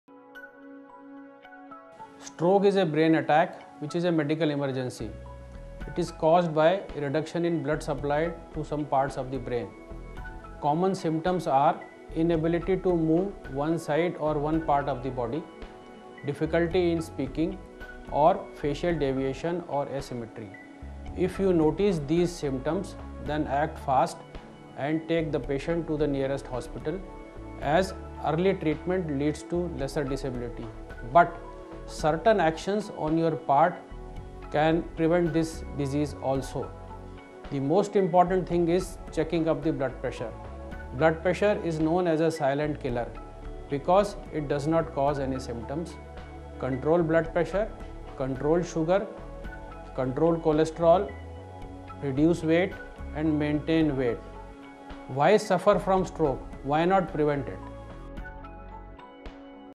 Dr. L. K. Malhotra -- 2
Stroke awareness message by Dr.L.K.Malhotra for SAFE T Initiative